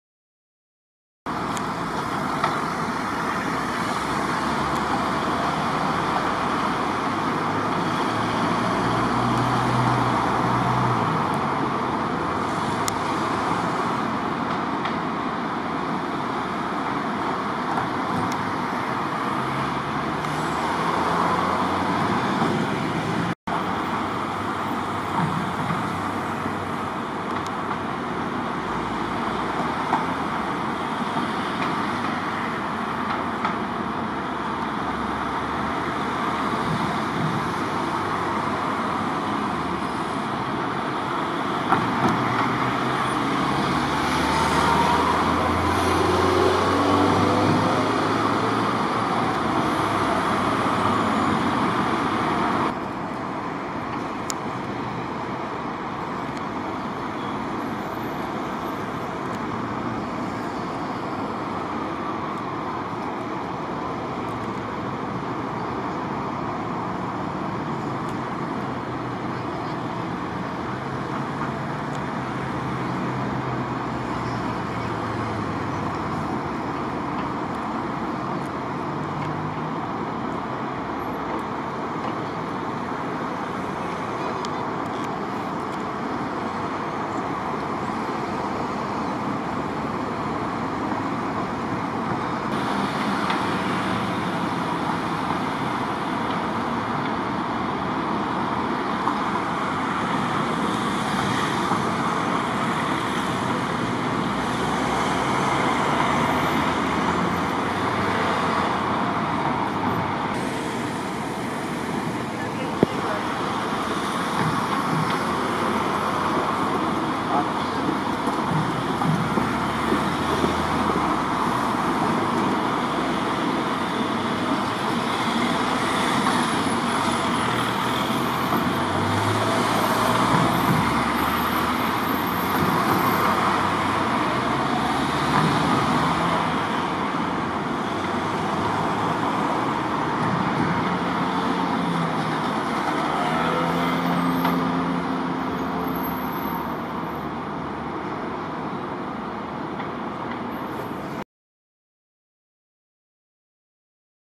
Nutria europea/Lontra/Eurasian otter, Lutra lutra, alimentándose al anochecer (río Lérez)
Una nutria (Lutra lutra) se pasea pescando a buen ritmo por la orilla urbana del río Lérez, Galicia. Enero 2019.
Fue filmada desde el puente de las corrientes y en sus inmediaciones, ya anocheciendo. Se observa como en breve espacio de tiempo captura varios ejemplares de lisa o múgil (Mugil spp), y aprovecha los recodos que le brindan las rocas para su captura. Suelen ser más activas al final del día.
Las nutrias son habituales en el Lérez, incluso en su tramos más urbanos (como se aprecia en el vídeo). Cada vez son más frecuentes en ambientes urbanos a los que parecen adaptase relativamente bien.